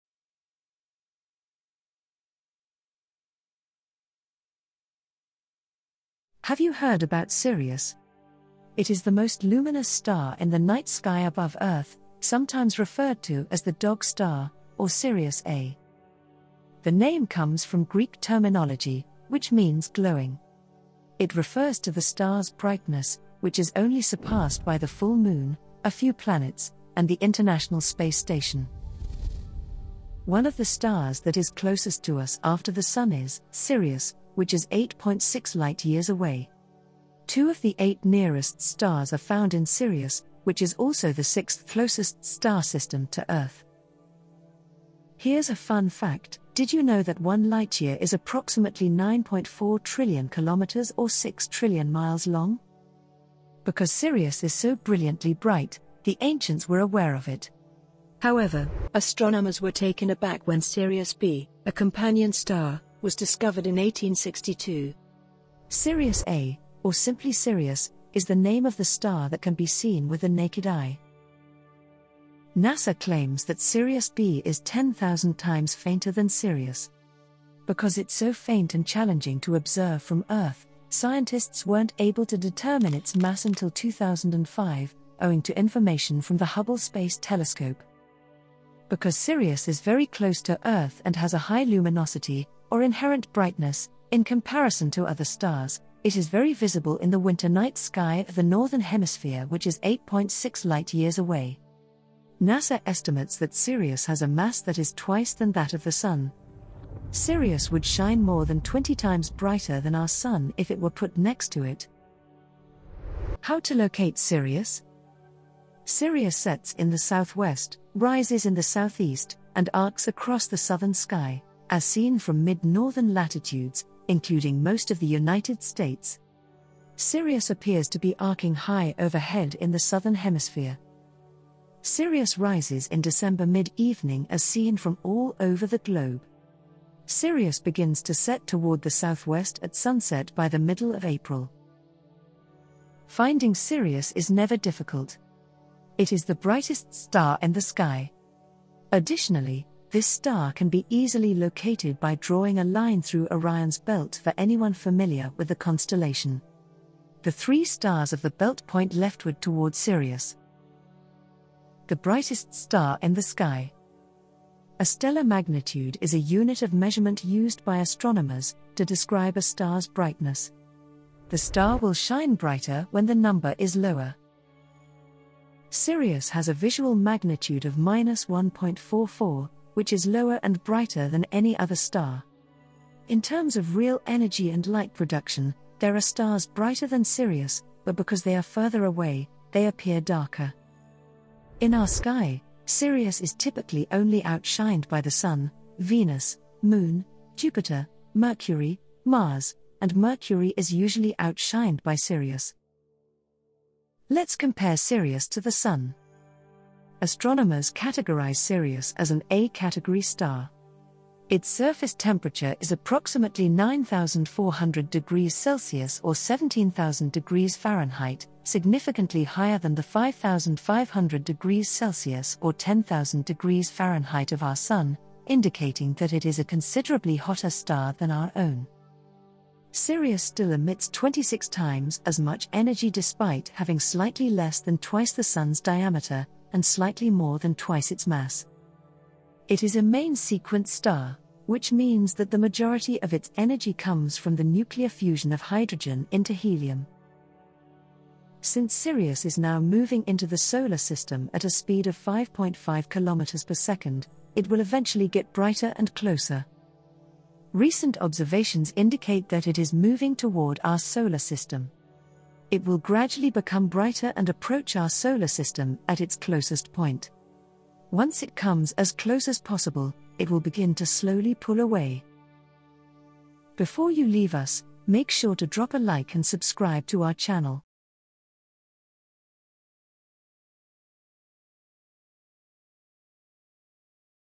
What Do We Know About Sirius?
Did you know that Sirius is the brightest star seen from the night sky? Not just this, it is about twice the size of the Sun but 26times brighter than the Sun! To kno w more about Sirius, why don't you keep watching?